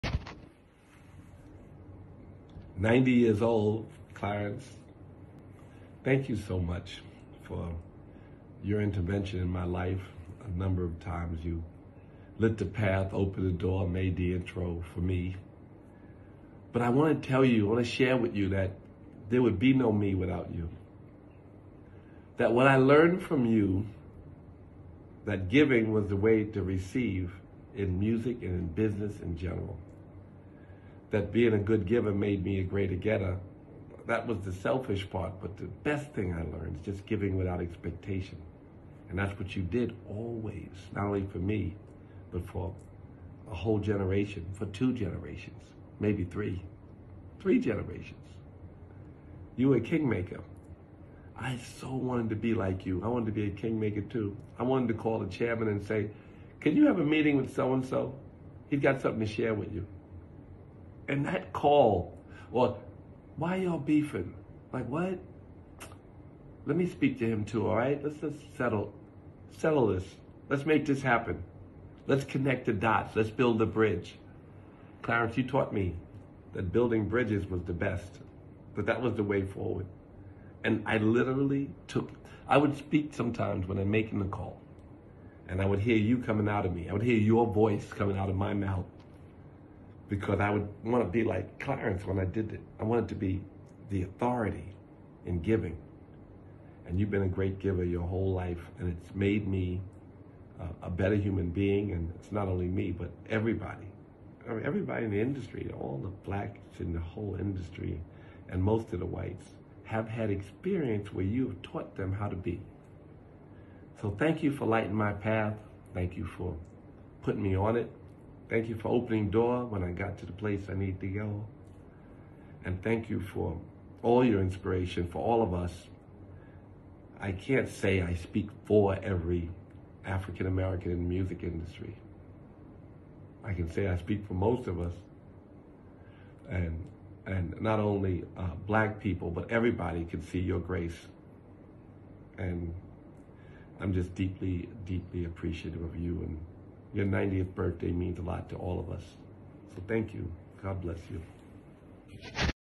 To the kingmaker on his birthday......Clarence Avant 90 years of love
The Music industry owes you so much.  I'm so lucky to have had you and still have you as an inspiration and a guiding light!! ..We the benefactors of your presence on earth ,honor and celebrate you. The true Godfather of the music industry ❤️